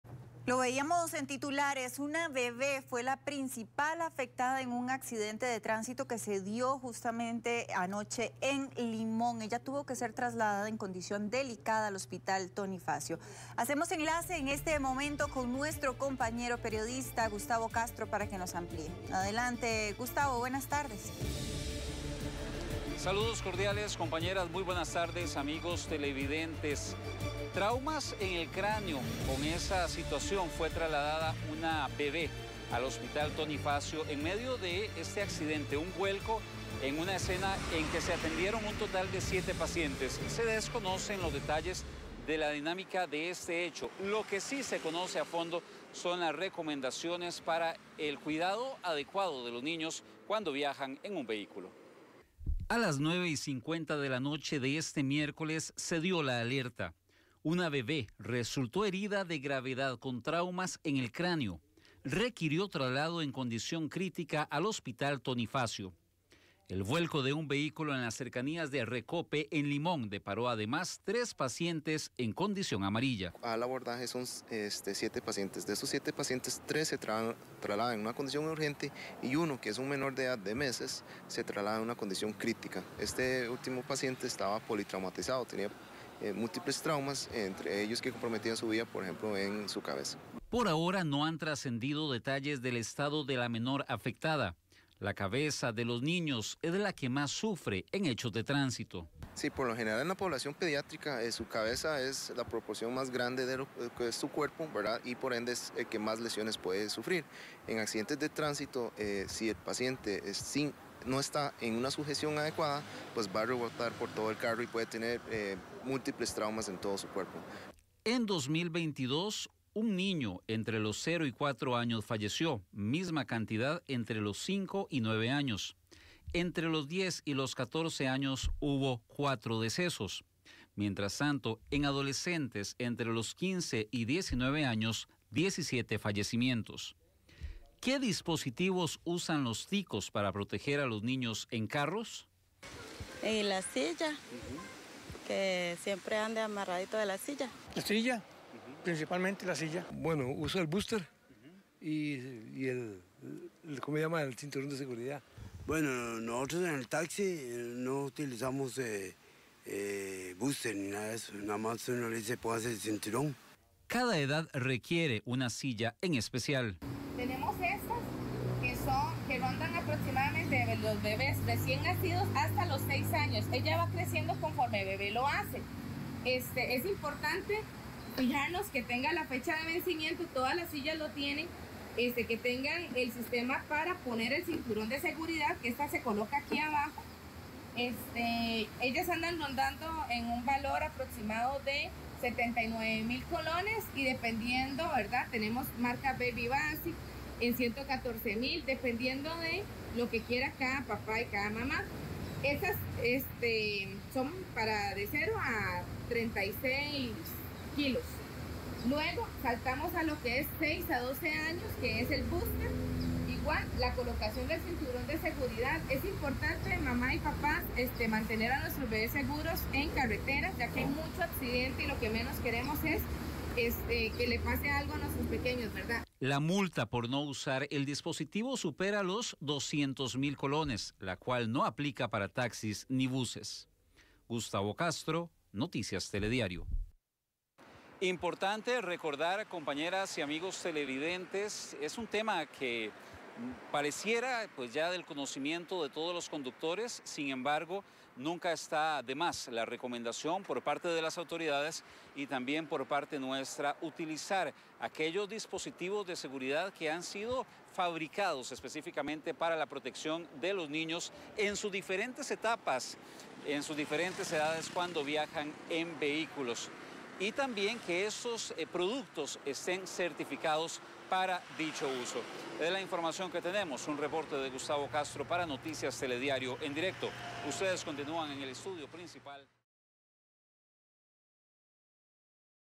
Bebé resulta herida tras vuelco de un vehículo en Limón
Tras el accidente la menor presentó múltiples traumas en su cabeza.